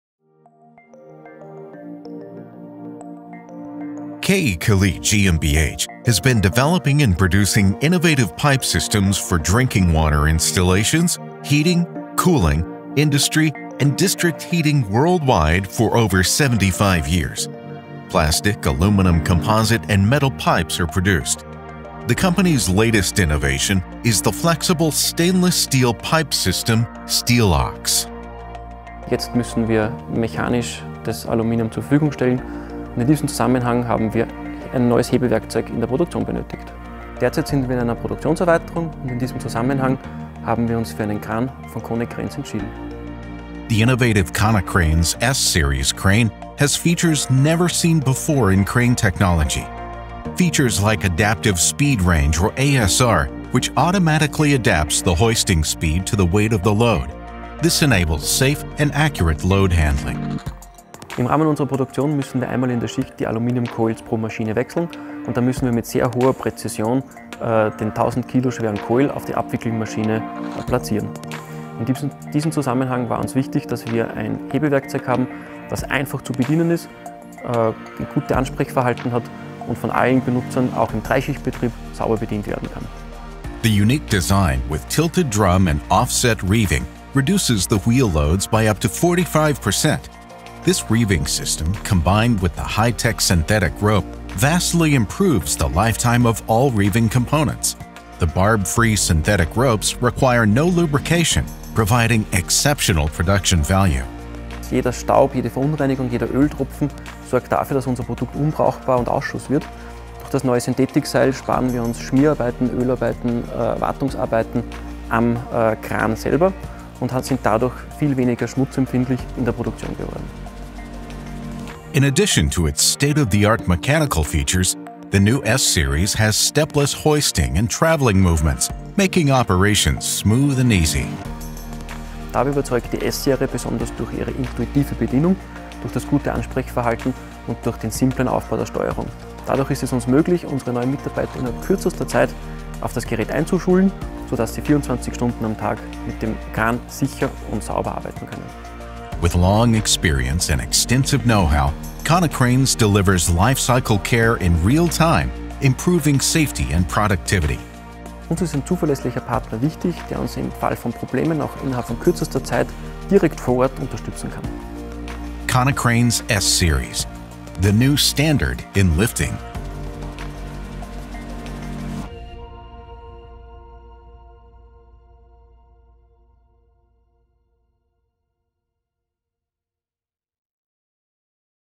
KE KELIT improves load handling accuracy and efficiency with Konecranes S-series crane.
Konecranes delivered new S-series crane for KE KELIT in Linz on the Danube, Austria in 2020. The innovative Konecranes S-Series crane with stepless travelling and hoisting movements together with ASR feature make load handling more accurate and efficient. The crane is also equipped with a state-of-the-art synthetic rope requiring no lubrication. This makes the S-series crane an optimal choice for the processes where cleanliness is vital.